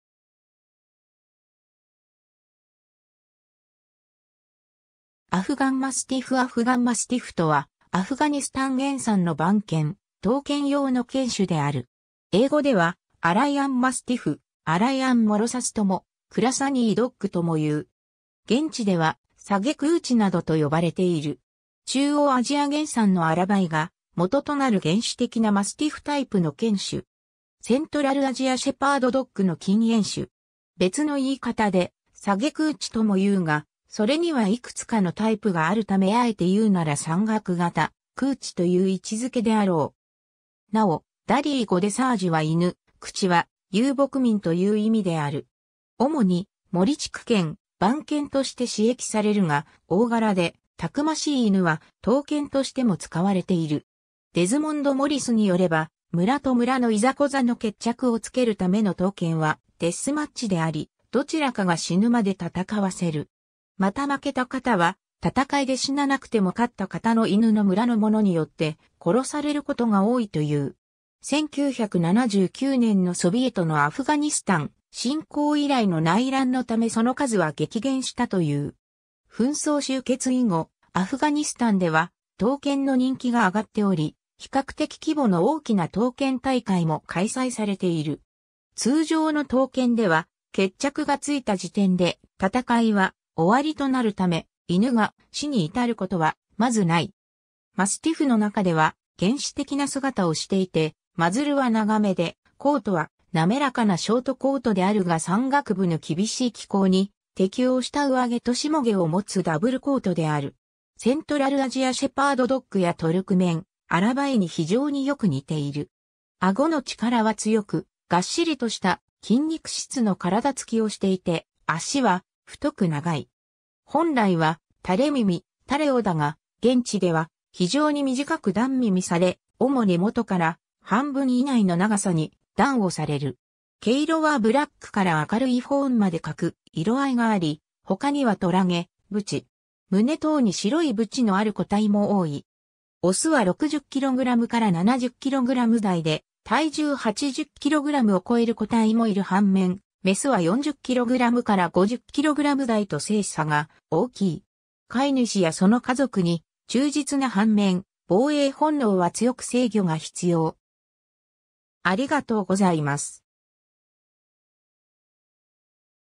アフガン・マスティフ
アフガン・マスティフ アフガン・マスティフ（英：Afghan Mastiff）とは、アフガニスタン原産の番犬・闘犬用の犬種である。
英語ではアライアン・マスティフ（英：Aryan Mastiff）、アライアン・モロサス（英：Aryan Molossus）とも、Khurasani Dogとも言う。
現地ではサゲ・クーチ（Sage kuchi）などと呼ばれている。
 中央アジア原産のアラバイがもととなる原始的なマスティフタイプの犬種。
セントラル・アジア・シェパード・ドッグの近縁種。
別の言い方でサゲ・クーチ（Sage kuchi＝クーチ犬）とも言うが、それには幾つかのタイプがあるため敢えて言うなら山岳型クーチと言う位置づけであろう。
なお、ダリー語でsageは犬、kuchi(kuci)は遊牧民と言う意味である。
 主に護畜犬、番犬として使役されるが、大柄で逞しい犬は闘犬としても使われている。
デズモンド・モリスによれば、村と村のいざこざの決着をつけるための闘犬はデスマッチであり、どちらかが死ぬまで闘わせる。
また負けた方は戦いで死ななくても勝った方の犬の村の者によって殺される事が多いという。
 1979年のソビエトのアフガニスタン侵攻以来の内乱のためその数は激減したという。
 紛争終結以後、アフガニスタンでは闘犬の人気が上がっており、比較的規模の大きな闘犬大会も開催されている。
通常の闘犬では決着がついた時点で戦いは終わりとなるため、犬が死に至る事はまず無い。
 マスティフの中では原始的な姿をしていて、マズルは長めでコートはなめらかなショートコートであるが山岳部の厳しい気候に適応した上毛と下毛を持つダブルコートである。
セントラル・アジア・シェパード・ドッグやトルクメン・アラバイに非常に良く似ている。
顎の力は強く、がっしりとした筋肉質の体つきをしていて、脚は太く長い。
本来は垂れ耳、垂れ尾だが、現地では非常に短く断耳され、尾も根元から半分以内の長さに断尾される。
毛色はブラックから明るいフォーンまで各色合いがあり、他には虎毛、斑。
胸等に白い斑のある個体も多い。
雄は60kg〜70kg台で体重80kgを越える個体もいる反面、雌は40kg〜50kg台(勿論大柄な個体もいるが少ない)と性差が大きい。
飼い主やその家族に忠実な反面、防衛本能は強く制御が必要。